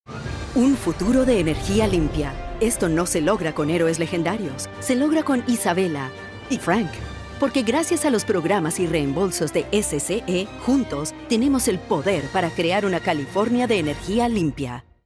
Creando una California de energía limpia
Ahora cada uno de nosotros tiene el poder para convertirse en un héroe cuando se trata de energía limpia. Empieza a ahorrar dinero ahorrando energía, en un esfuerzo por ayudar a crear una California de energía limpia.